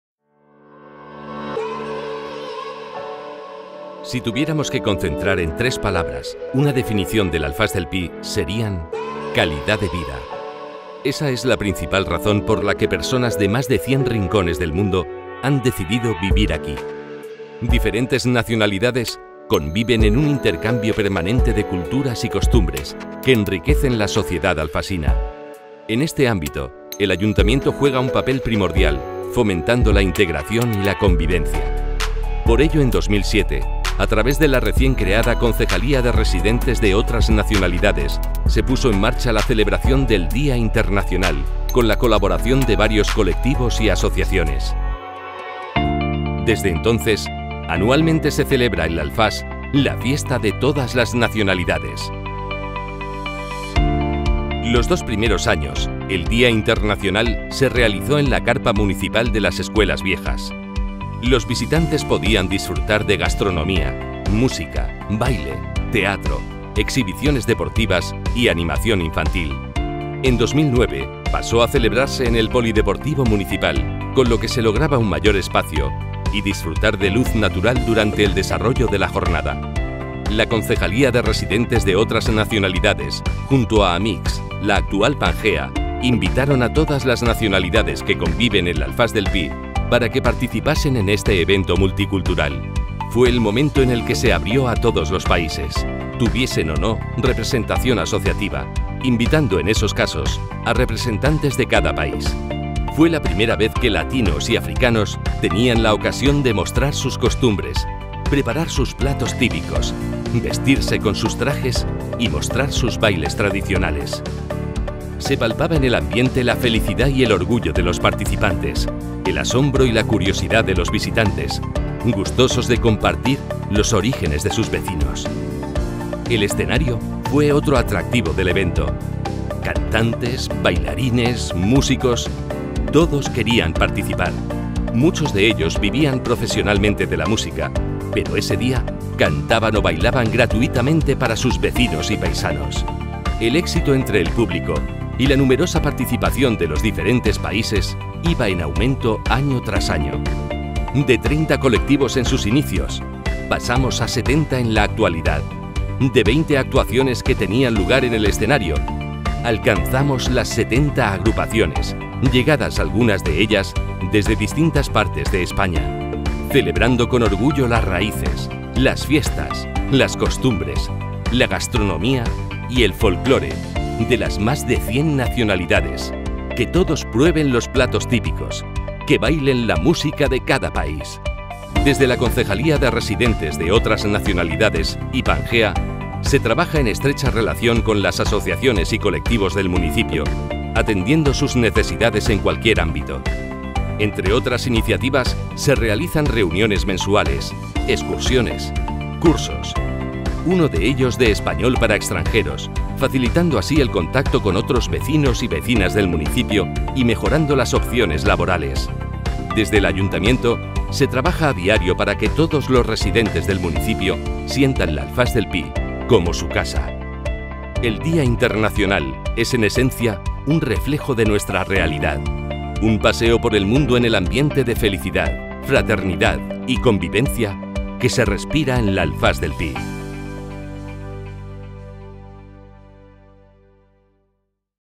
Día Internacional 2007-2019 lAlfàs 2020
Día Internacional de l’Alfàs, una iniciativa de carácter festivo y cultural, pionera en la Marina Baixa, puesta en marcha en su día por el Ayuntamiento en colaboración con las asociaciones y colectivos, para conmemorar algo que diferencia a esta localidad de otras poblaciones cercanas, y son los residentes que aquí conviven, de casi cien nacionalidades diferentes, y superan el 50 % del censo poblacional.